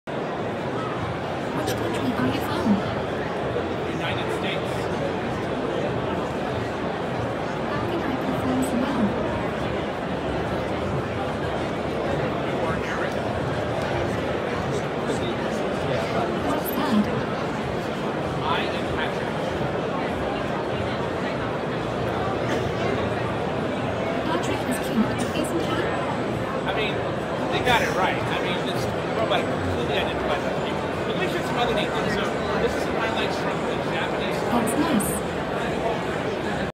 英語ペラペラ アンドロイド・アナウンサー at CES2020
日テレアナウンサーとしても有名な、アンドロイド・アナウンサーです。
もう、人間そっくりです。
扱いは、新人アナウンサー以上です。
衣装担当、メイクさんまでついてます。
J-StartUpの入り口にいて、通る人の注目をがっちり集めていました。

もちろんエリカは英語ペラペラ。
現在はまだ日本語と英語のバイリンガルですが、今中国語を学習中だそうです。